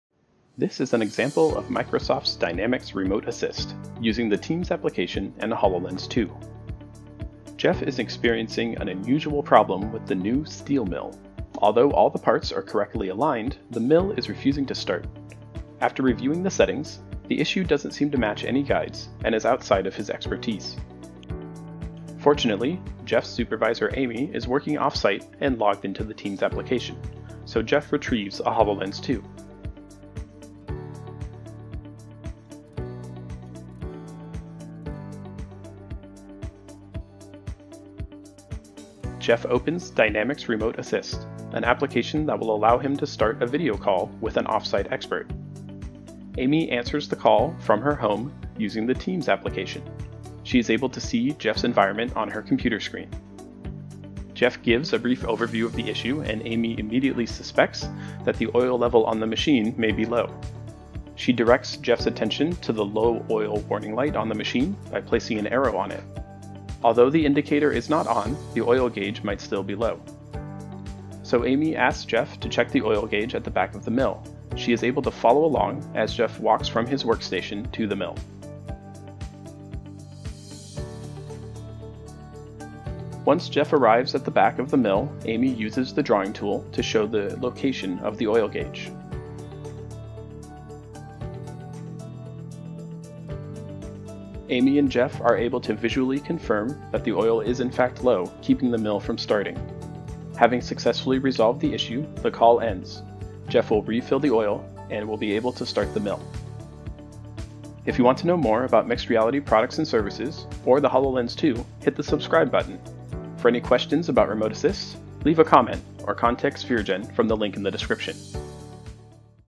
Microsoft Remote Assist - Ulbrich Manufacturing Case Study
Ulbrich is a manufacturing company located in Connecticut and South Carolina specializing in manufacturing metal. See how a remote employee assists an on-site employee on the manufacturing shop floor using the Microsoft HoloLens 2 and Microsoft Teams. Remote guidance enables technicians to solve downtime problems quickly and efficiently, with no travel required.